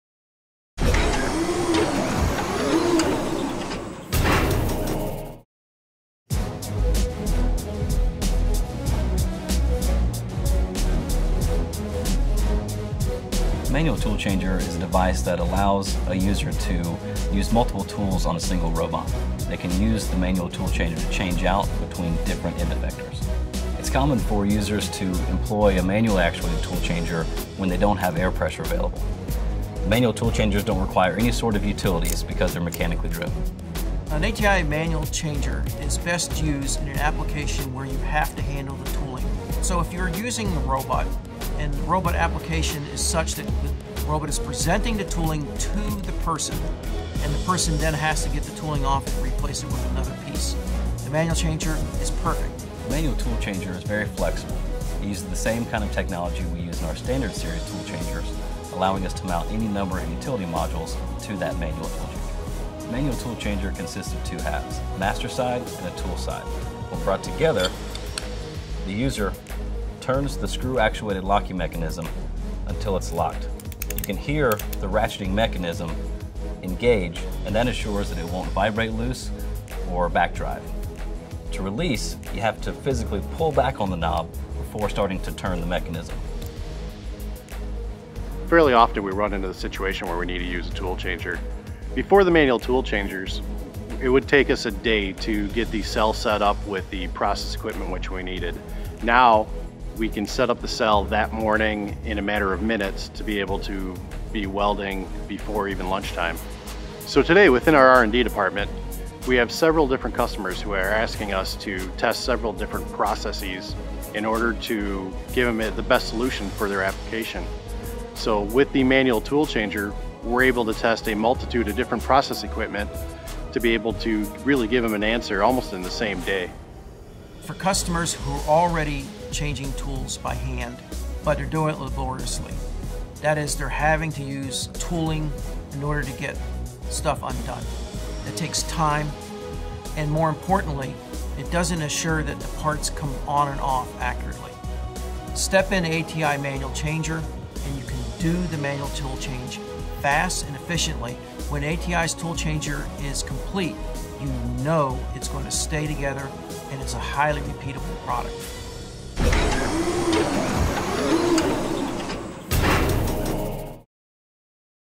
Why ATI Manual Tool Changers?
This video explains what an ATI Manual Tool Changer is and how it allows tooling and utilities to be quickly changed by hand. ATI Industrial Automation is the world leader in robotic end-effectors and robot arm tooling. for more information.